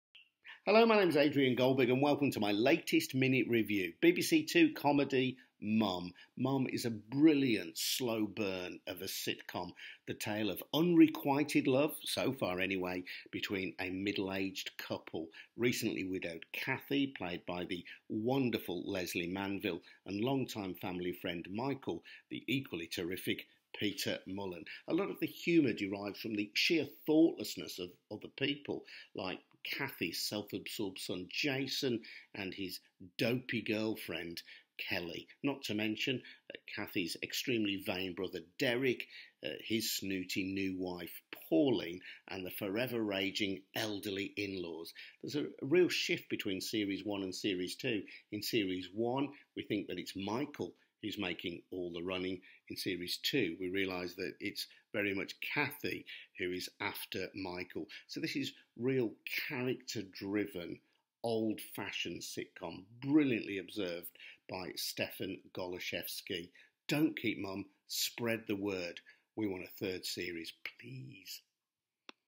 MUM (Adrian Goldberg's Minute Review)
Adrian Goldberg's Minute Review of BBC 2 sitcom Mum, starring Lesley Manville and Peter Mullan, written by Stefan Golaszewski